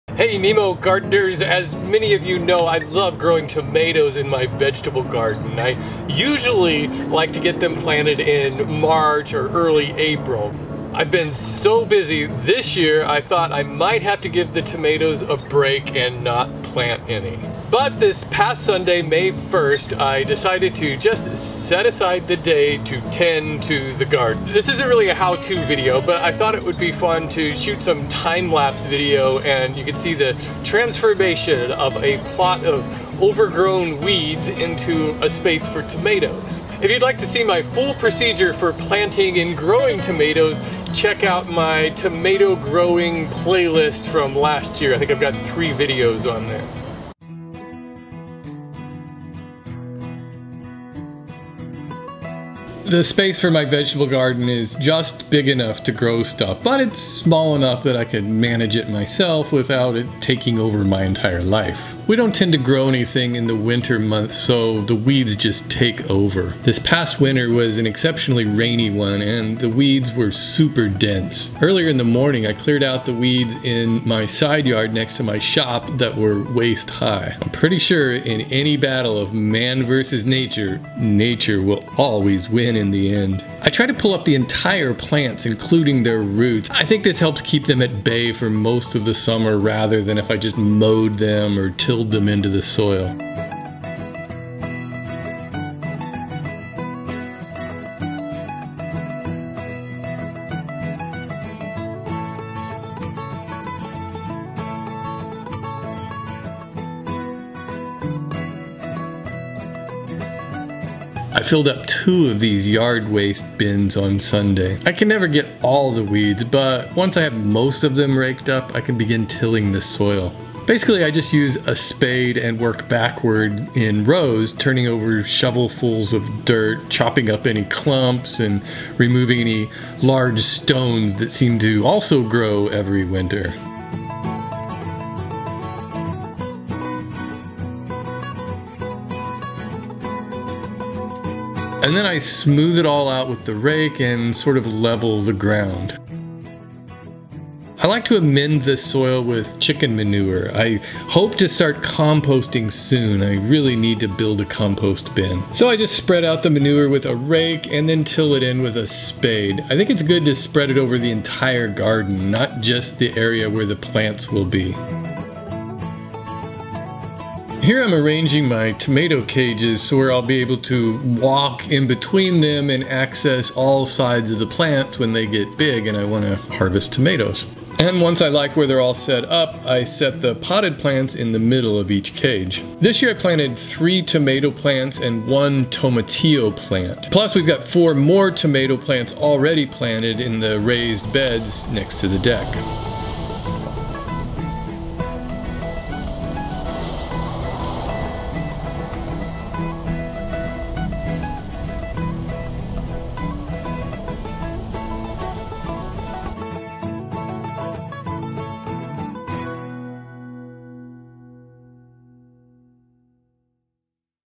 TIME-LAPSE: Prepping and Planting my Garden
What could be more fun than sitting back and watching a guy pulling weeds and shoveling dirt?! TIME-LAPSE footage of a guy pulling weeds and shoveling dirt, that's what! I hope you enjoy my little movie.

For Mere Mortals is a trademark of